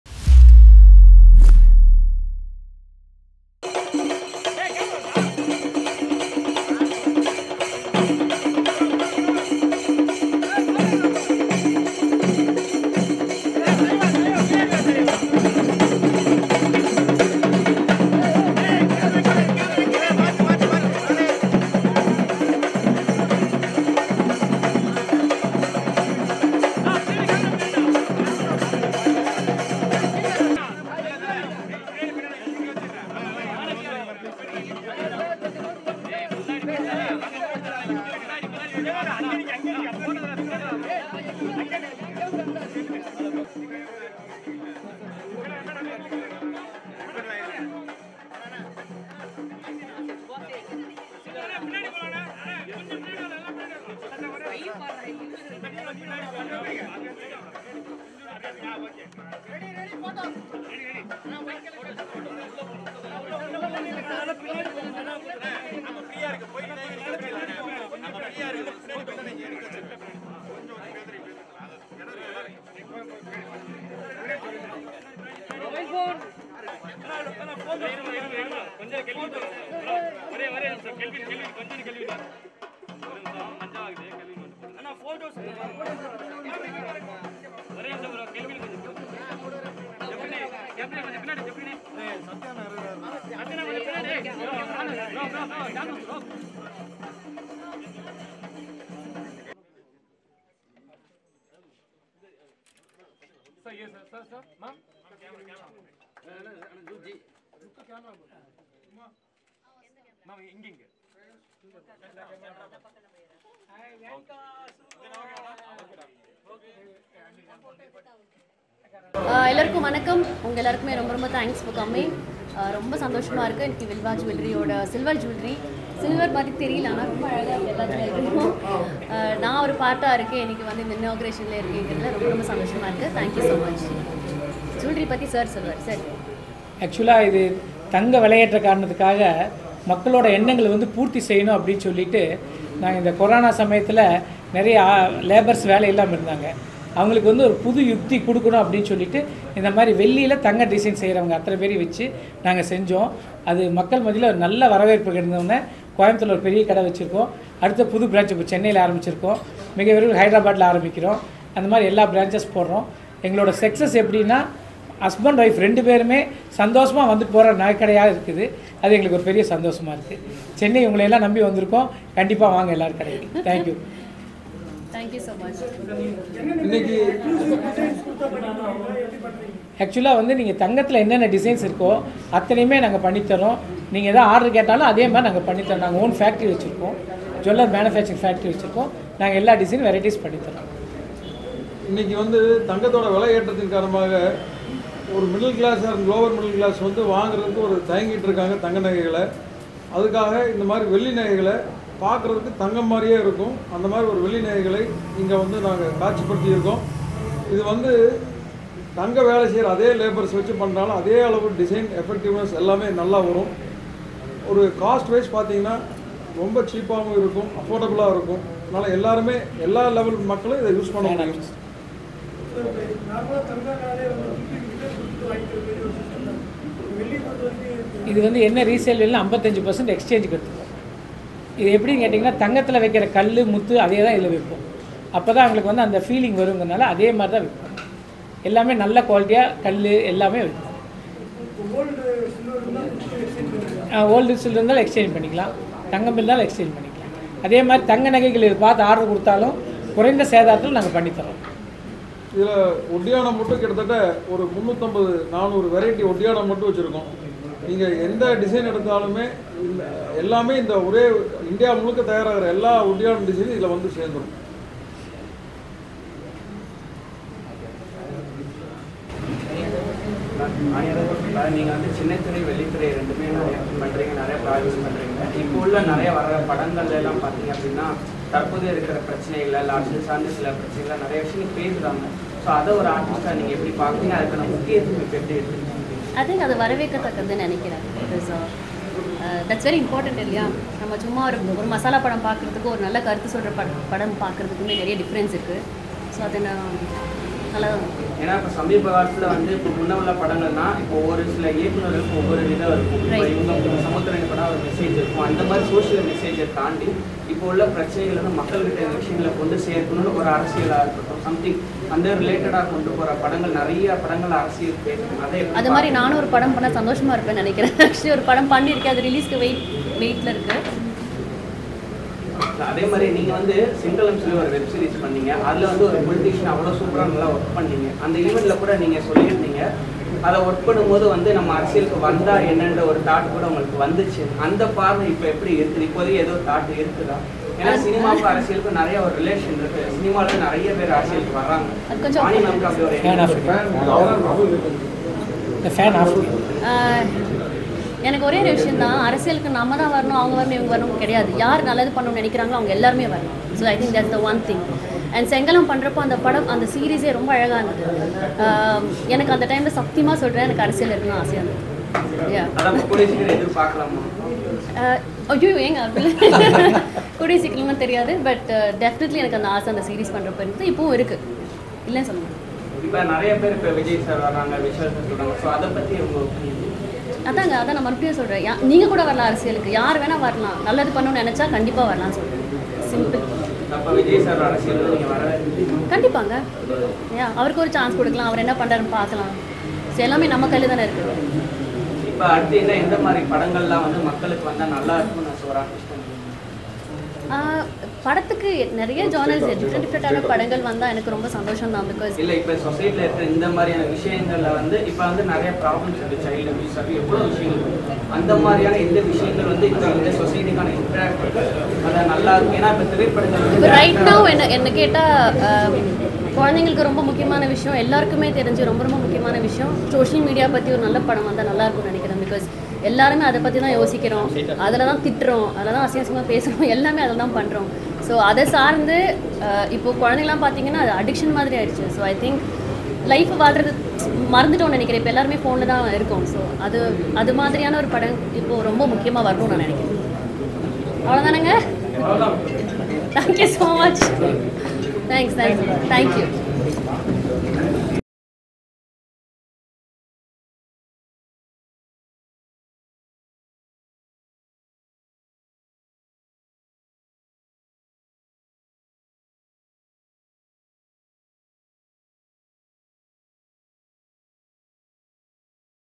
விஜய் அரசியலுக்கு Vani Bhojan பதில் ! |Tamilnadu's First & Largest Silver Jewelry Boutique
Make a Head Turn to Anna Nagar

Grand Opening Ceremony of Tamilnadu's First & Largest Silver Jewelry Boutique Vilvajewels In Anna nagar!!!

Inaugurated by Gorgeous @vanibhojanoffl